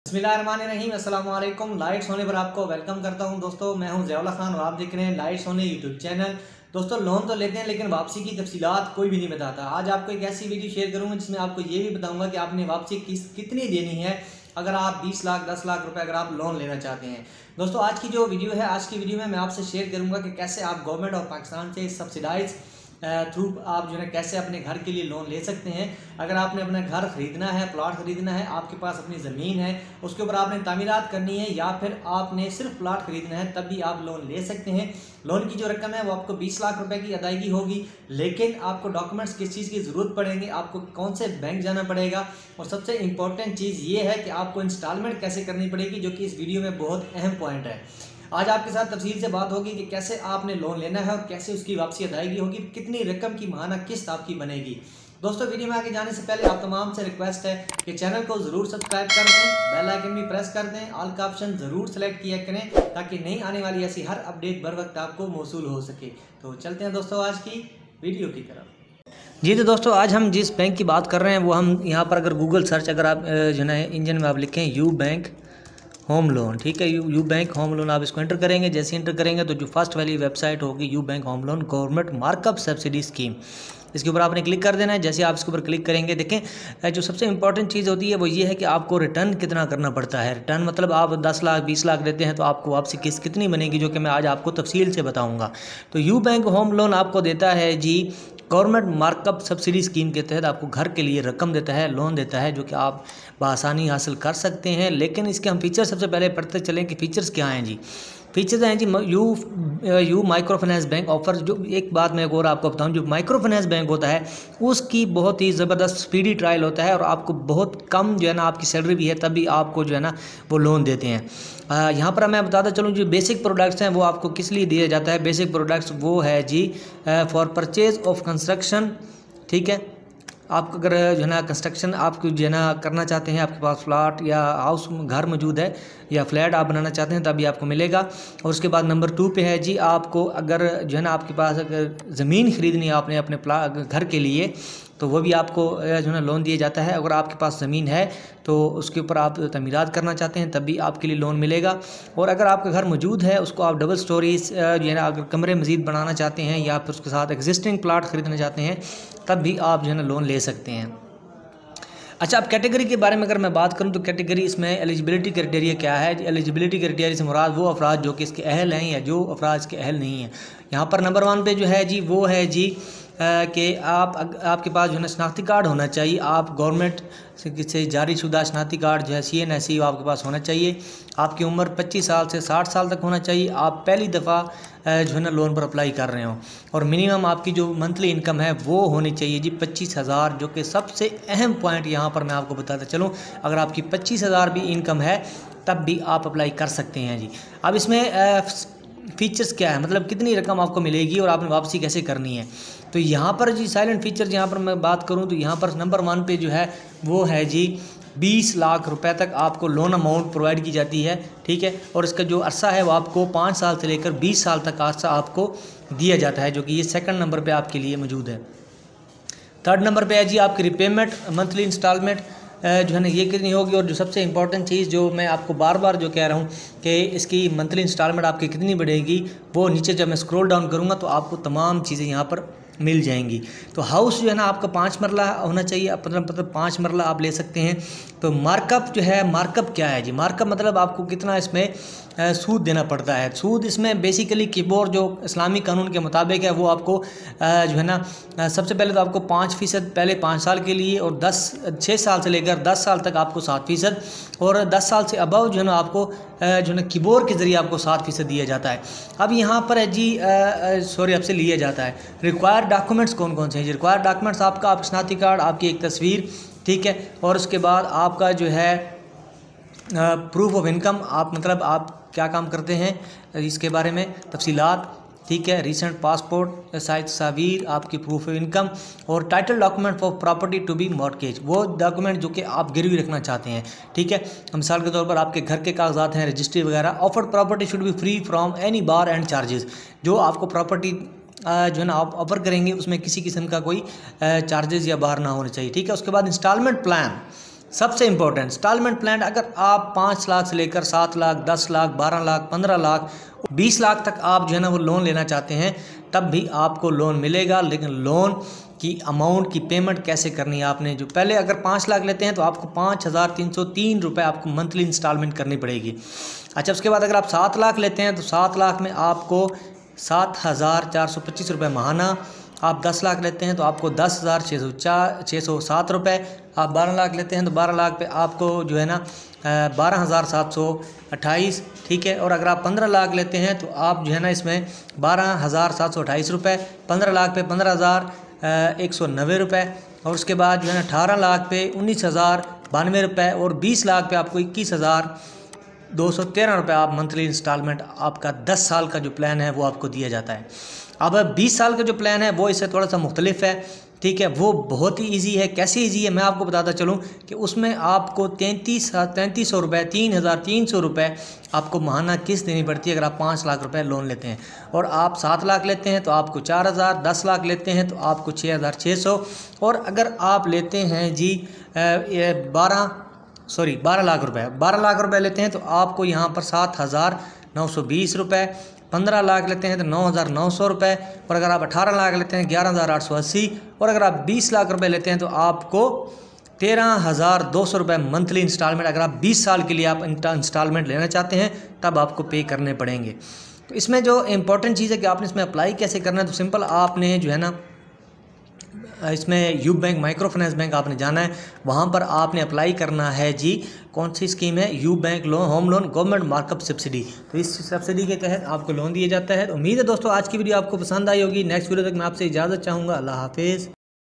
UBank loan scheme 2021[ U bank home loan ] How to get housing loan from UBank | lightsonly
Aslamu alaikum
My dear friends in this video you will know about Ubank house loan and more about this loan we can say that  loan is a money you borrow for just about to build your dream home or purchase of your plot if you need to construct new home or plot and for any purpose you really need it.
Today we are talking about UBank loan scheme 2021 | U bank home loan |How to get housing  loan from UBank | lightsonly.
 if you want to borrow money from ubank so you are at the right place because I have given you all the details of borrowing money from ubank in this video. You will learn about the documents required and guarantee as well as you will know about how much money you will be given in this scheme and how to return the borrowed money from Ubank loan scheme 2021 , and loan scheme is best option for you get loan from it.